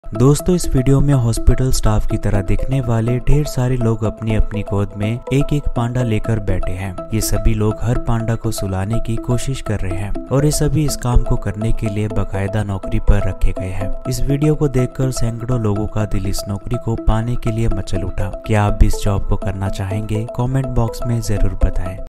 Everyone ready for the job of putting the Panda to sleep
Viral Video Everyone ready for the job of putting the animal to sleep.